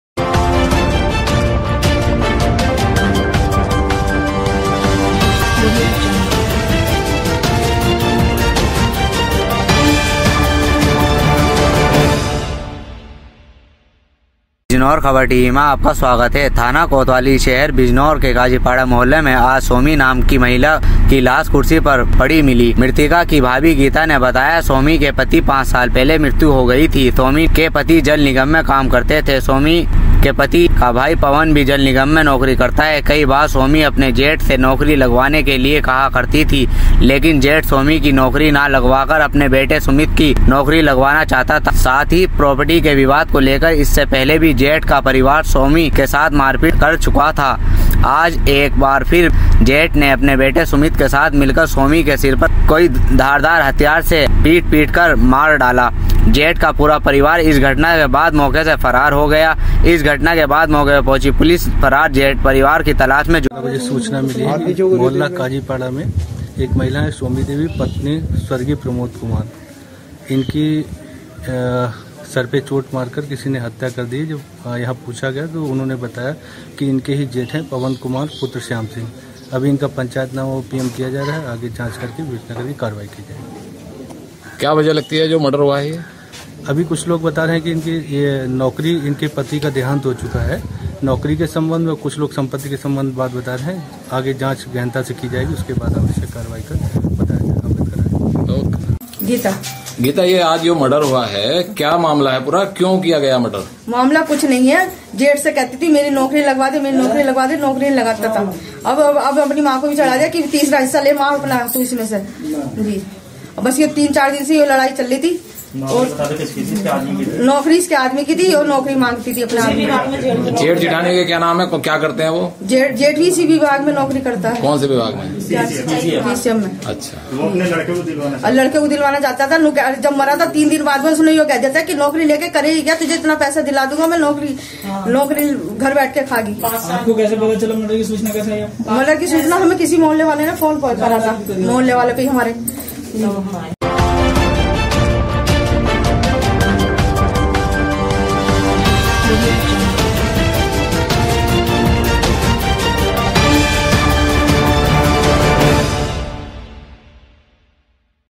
BIJNOR KHABAR || बिजनौर काजीपाड़ा मोहल्ले में नौकरी के लालच मे जेठ ने भाभी की हत्या
अगर आपको कोई खबर भेजनी है तो इस नम्बर पर सम्पर्क करें बिजनौर ख़बर टीवी हेड ऑफिस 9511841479
बिजनौर जेठ और जेठानी पर भाई की पत्नी की हत्या का आरोप लगा है।मृतिका की बहन ने महिला के जेठ और जेठानी सहित बेटे और बेटी पर प्रॉपर्टी और नौकरी के विवाद को लेकर हत्या करने का आरोप लगाया है।महिला की हत्या सिर पर भारी लोहे के सामान से वार करके की गई है। सूचना मिलने पर मौके पर पहुंची पुलिस ने शव का पंचनामा भरकर शव को जिला अस्पताल पोस्टमार्टम हाउस भेज दिया है।इस घटना के बाद जेठ का पूरा परिवार घटनास्थल से फरार हो गया है।मृतिका के परिजनों द्वारा जेठ के पूरे परिवारों के खिलाफ हत्या की तहरीर थाने में दी गई है।
थाना कोतवाली शहर बिजनौर के काजीपाड़ा मोहल्ले में आज सोमी नाम की महिला की लाश कुर्सी पर मृतिका के परिजनों को पड़ी मिली।मृतिका की भाभी गीता ने बताया कि सोमी के पति प्रमोद कुमार की 5 साल पहले मृत्यु हो गई थी। सोमी के पति जल निगम में काम करते थे।सोमी के पति का भाई पवन भी जल निगम में नौकरी करता है ।कई बार सोमी ने अपने जेठ से नौकरी लगवाने के लिए कहा था लेकिन जेठ सोमी की नौकरी ना लगवाकर अपने बेटे सुमित की नौकरी लगवाना चाहता था। साथ ही घर की प्रॉपर्टी के विवाद को लेकर इससे पहले भी जेठ का परिवार सोमी के साथ मारपीट कर चुका था।आज एक बार फिर से जेठ ने अपने बेटे सुमित के साथ मिलकर सोमी के सिर पर किसी भारी लोहे  के सामान से वार कर उसकी हत्या कर दी और जेठ का पूरा परिवार इस घटना के बाद मौके से फरार हो गया है।इस घटना को लेकर सीओ कुलदीप गुप्ता ने बताया कि मौके पर पहुंची पुलिस फरार जेठ के परिवार की तलाश में जुट गई है।प्रॉपर्टी और नौकरी के विवाद में इस हत्या को अंजाम दिया गया है।पुलिस जल्द ही आरोपी के परिवार को पकड़ लेगी।
बाईट।कुलदीप गुप्ता।सीओ बिजनौर
बाईट।गीता।मृतिका की बहन